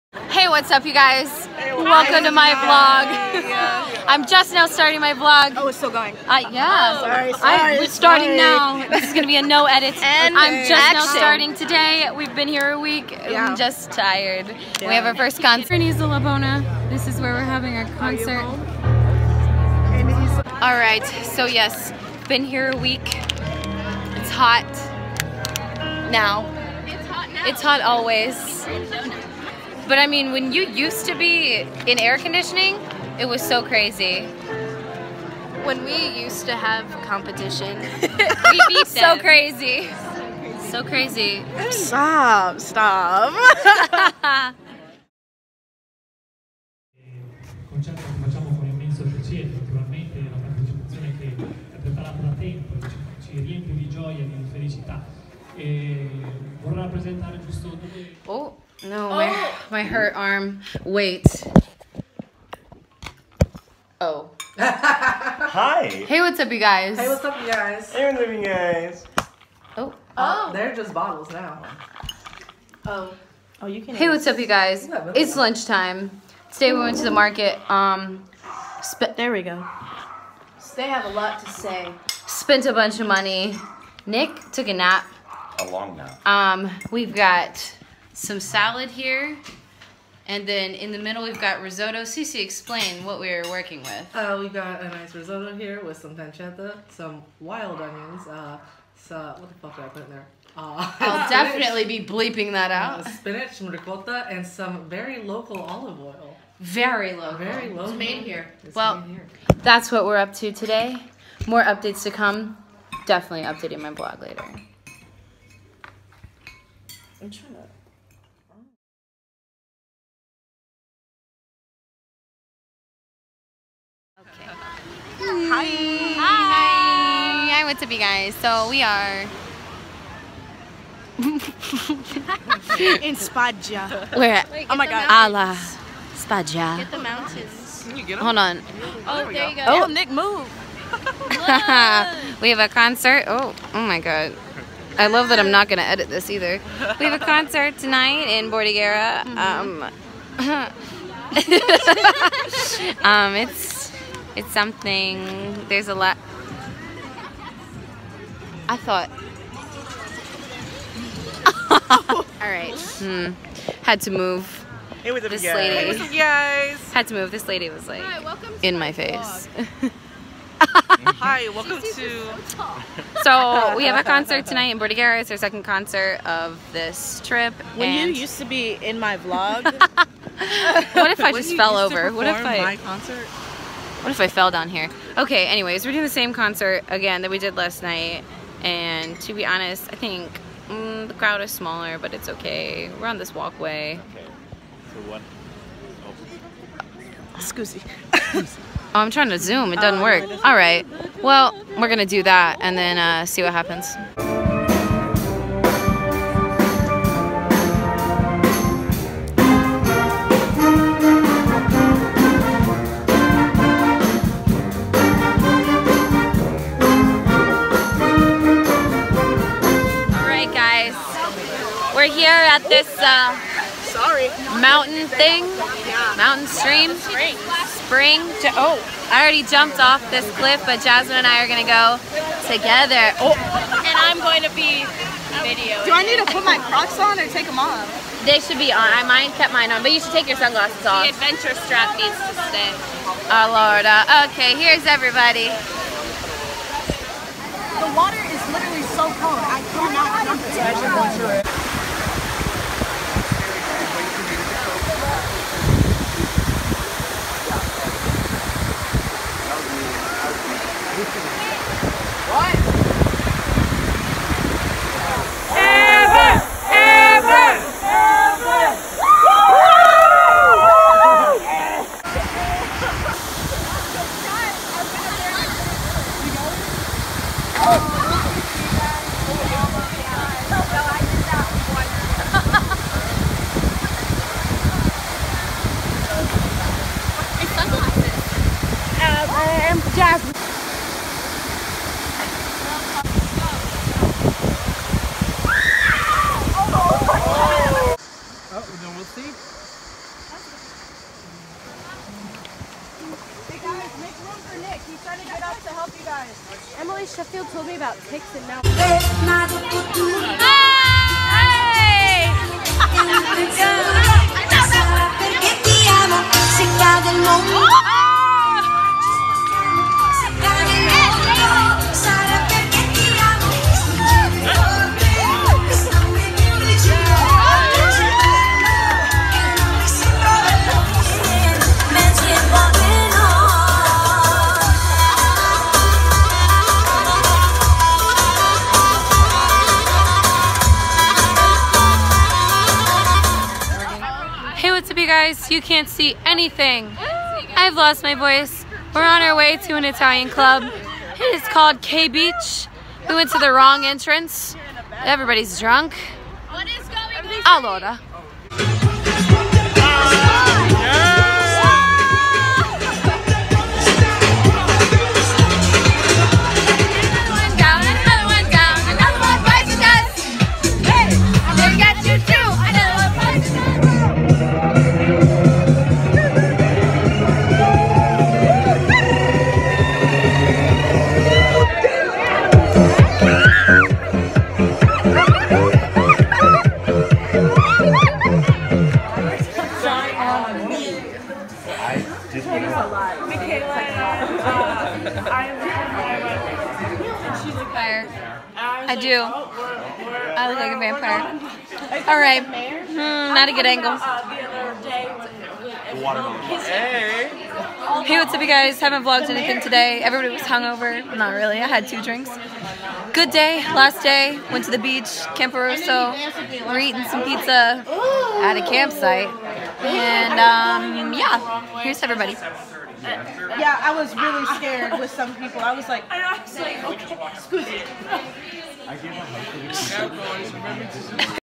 ISOLABONA DAYZ
a completely unedited video of what went on the week in isolabona. i thought that i was going to be recording everything and photographing everything but that isn’t what happened. i was so enamored by the whole place that i had to force myself to record things because i didn’t want to miss anything happening in real time. with that being said, i hope you all enjoy this quick peek into what we got up to in isolabona that second week of July.

NEW VIDEOS WEEKLY(ish)